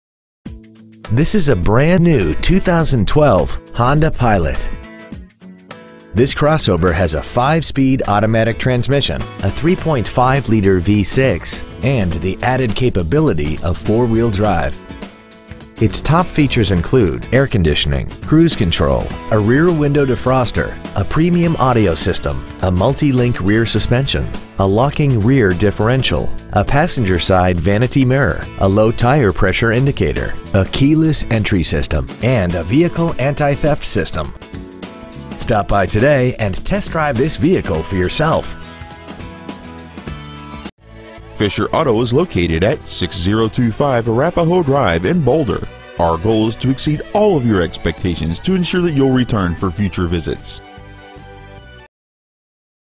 2012 Honda Pilot Denver CO
Year : 2012
 Make : Honda
 Model : Pilot
 Engine : gas v6 3.5l/212
 Trans . : automatic
 Exterior : orange
 Miles : 7
 Interior : tan
 Stock : 123602

 2012 Honda Pilot Boulder CO